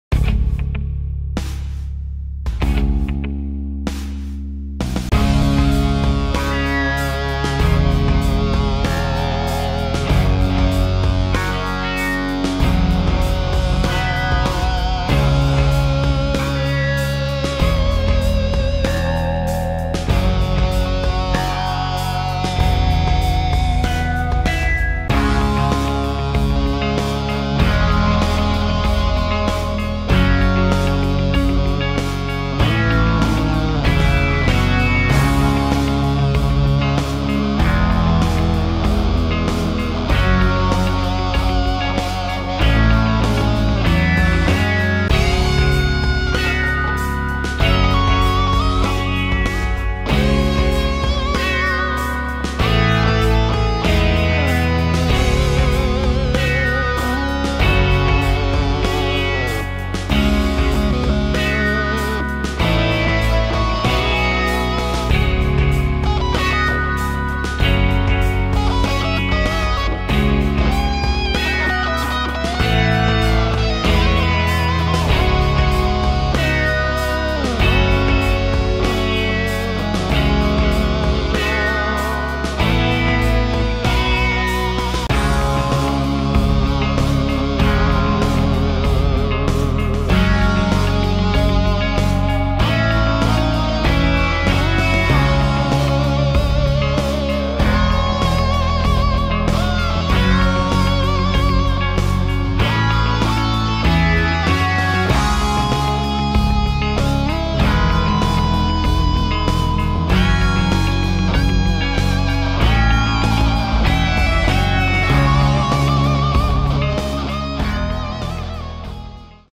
Jupiter in love- Mars vs Jupiter
My vision is that Makoto is in love with Rei. Makoto dosen't know that Rei is a soldier and sees her transform one day. Makoto then confesses her feeling to Mars, who in turn dosent know Makoto is a solider aswell, turns her down and trying to push her away for knowing her identity... then they find eachother in the end